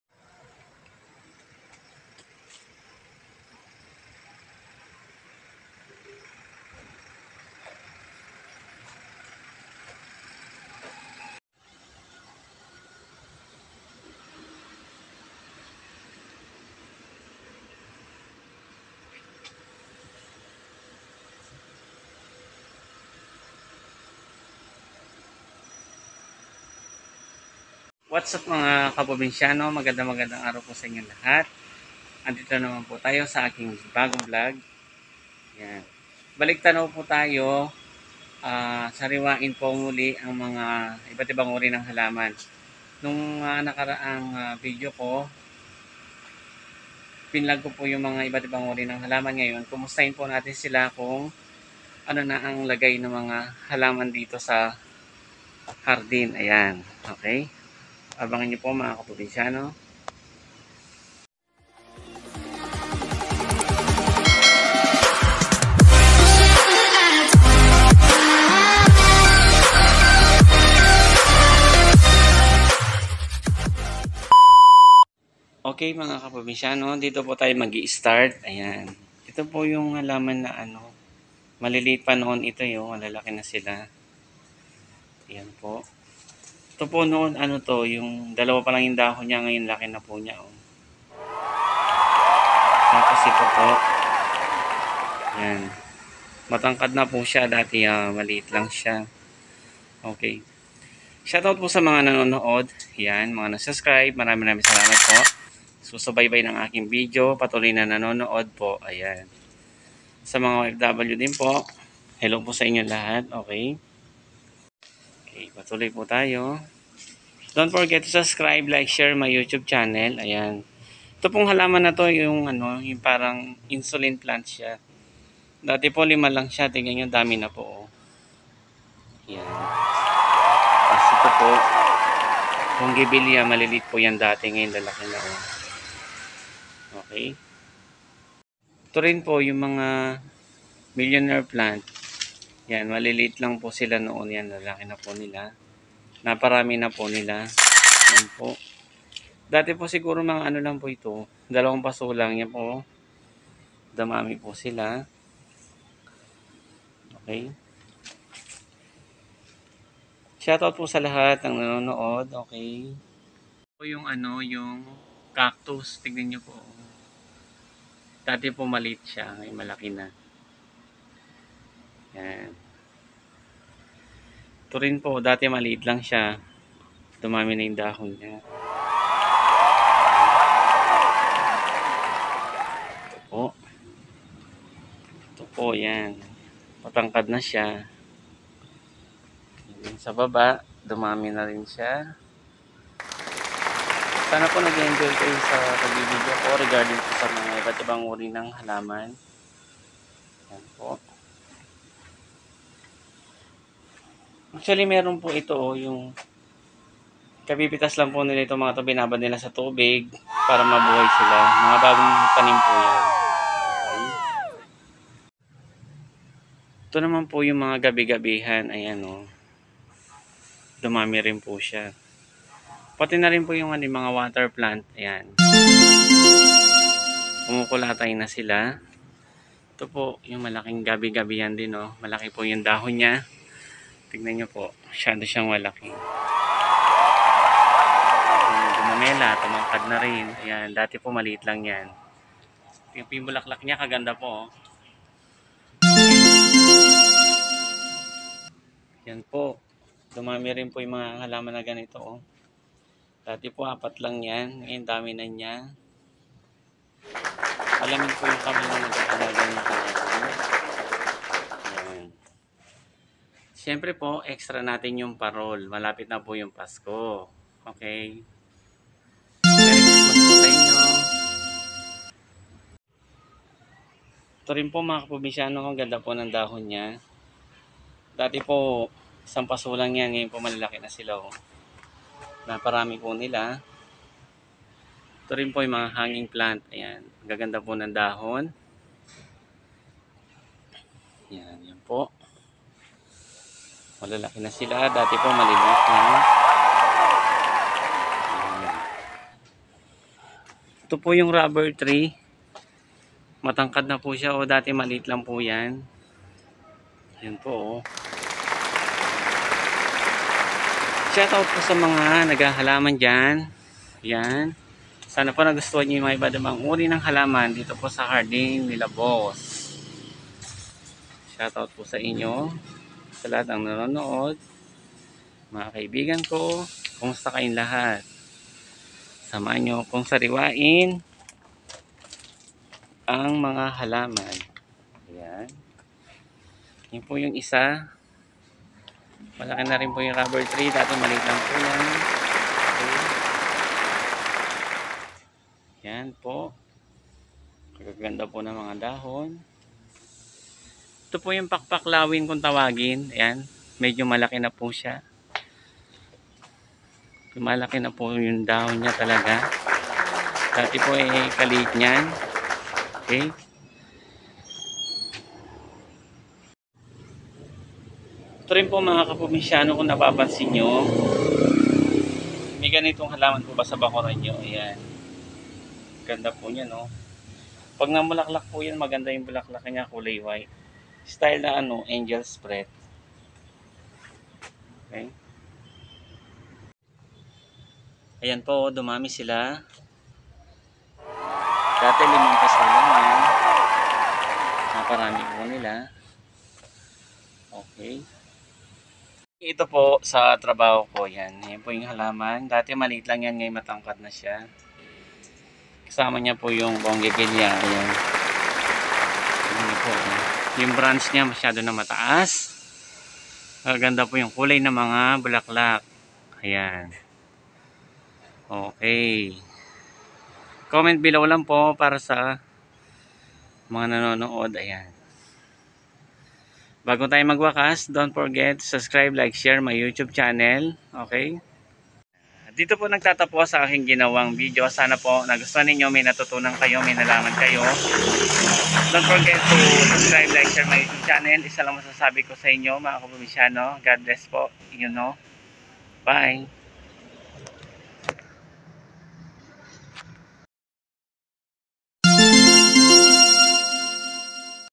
pagbabalik tanaw, at ating sariwain Ang mga iba't iBang URI Ng halaman; marlonguinsatao vlog
nakaka egganyong maghalaman, Lalo na't nakikita mo silang lumalaki, masigla, at lumalabong at namumulaklak Ang mga ito.,
marlonguinsatao vlog